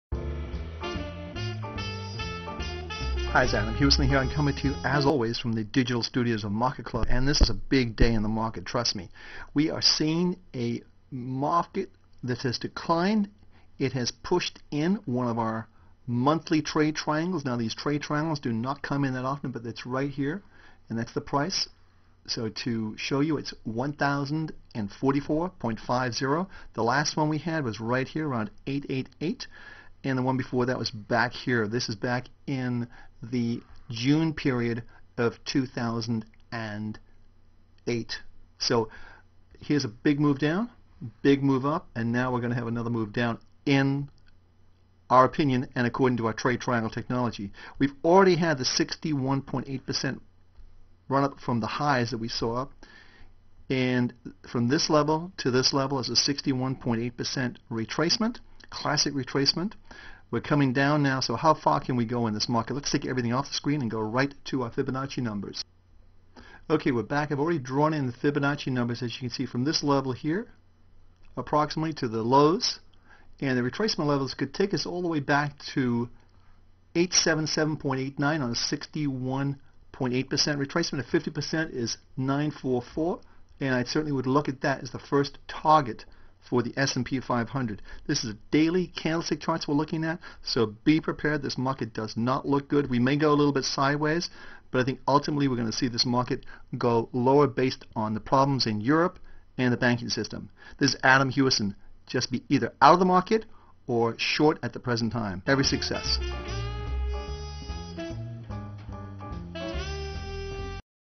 One year later, reality sets in
It's been just a little over a year since we had our first major buy signal for the S&P 500 at 888.70 on 5/4/09. Since that time, the S&P 500 has climbed approximately 61.8% from the lows that were seen in early March of '09 and the highs that were seen in October of '07.

We take our "Trade Triangle" technology very seriously and this signal today (5/25) at 1044.50 is our first major sell signal since 7/1/08 at 1,272.00 and should not be ignored.

There are a whole host of problems that are coming due around the world that will have negative consequences for the equity markets. The problems in Greece and Europe are well known and are likely to continue for the balance of the year. This is going to have a negative impact on markets in general.

In my new short video I show you exactly what I think is going to happen to the S&P 500 market and just how you can protect yourself if we are correct. As always our "Trade Triangles" will dictate all market action. At the present time all of our "Trade Triangles" are negative and pointing to the downside. This indicates that a very strong trend is in place and it likely to continue.

Many traders, especially younger traders, are unaware of how bear markets work. Bear markets tend to be demoralizing as they do not  have any strong and sustained rallies. They tend to erode as more and more traders become unnerved and throw in the towel.

I invite you to take a look at this new video with no registration and no charge.

Whether you agree, disagree, or just want to comment on this video, please do so on our Trader's Blog.

Adam Hewison
President, INO.com

Co-creator, MarketClub